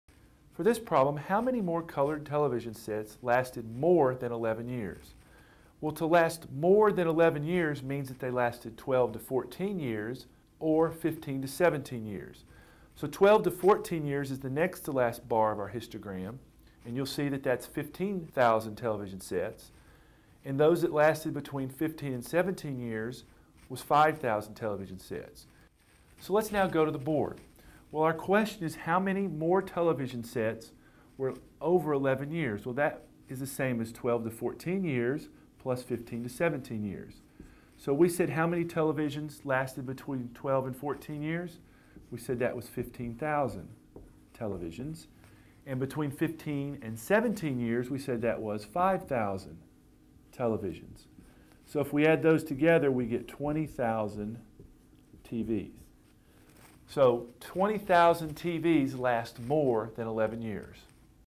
Chapter Test Prep - Ch 8, Exercise 19
An instructor walks you step-by-step through the solution to Exercise 19 in the Chapter 8 Test from the textbook Tobey Basic College Mathematics 9e.